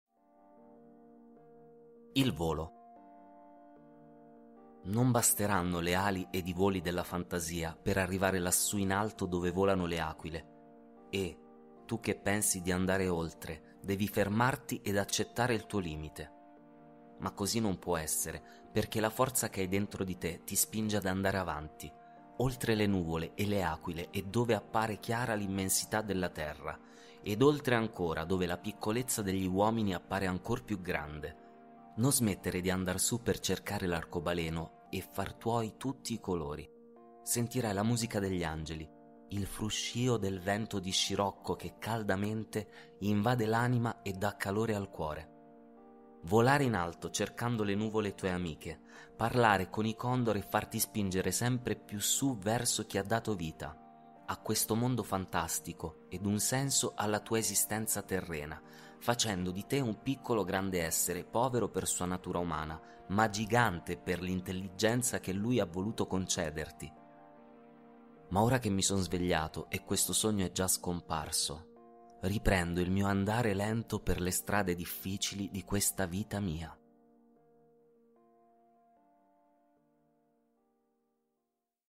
Brunello Esu il volo
Dopo aver trascorso 27 anni a Cagliari conseguendo la laurea in Economia e Commercio, da circa quarant’anni vive a Carbonia dove ha svolto l’attività di bancario. Scrive poesie da oltre 50 anni, ottenendo alcuni riconoscimenti nei vari concorsi di poesia ai quali ha partecipato. Il tema seguito è rivolto al quotidiano e a tutte le &hellip; Scrivi poesie? Partecipa al Concorso. Primo Premio 1500€